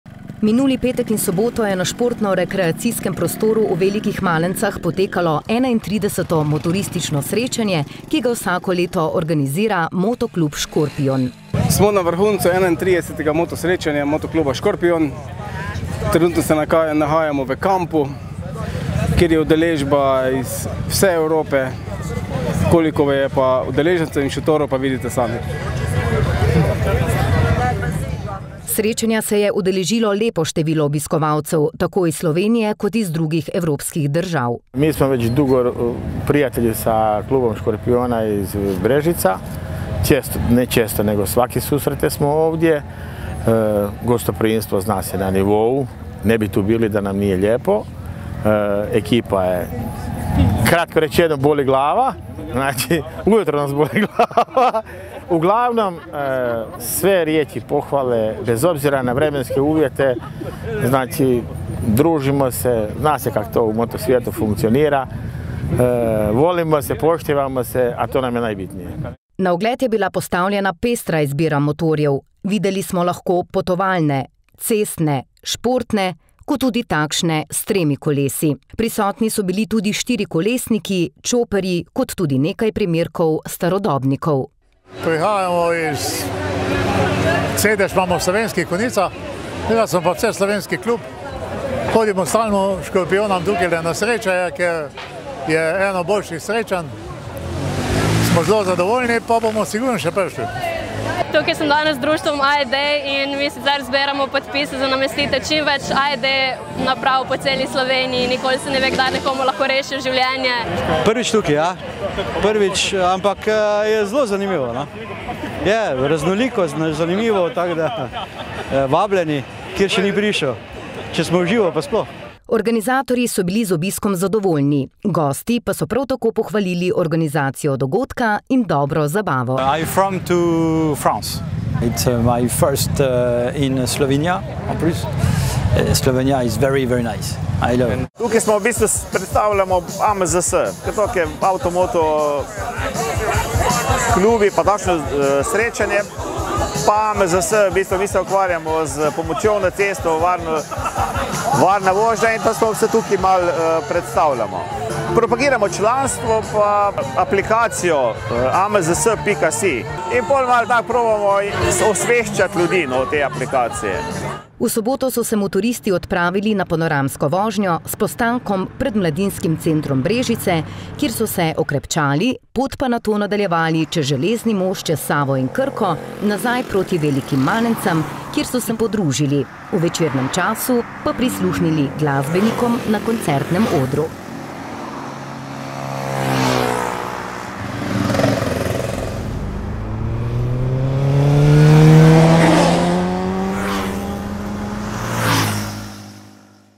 VELIKE MALENCE: 31. motoristično srečanje Moto kluba Škorpijon
Na športno – rekreacijskem prostoru v Velikih Malencah je v petek in soboto, 18. in 19. avgusta 2023, potekalo 31. motoristično srečanje Moto kluba Škorpijon.